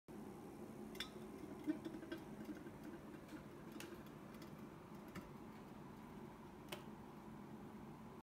太陽電池の実験
太陽電気の発電を体験する実験キット、エネルギーの仕組みを知る。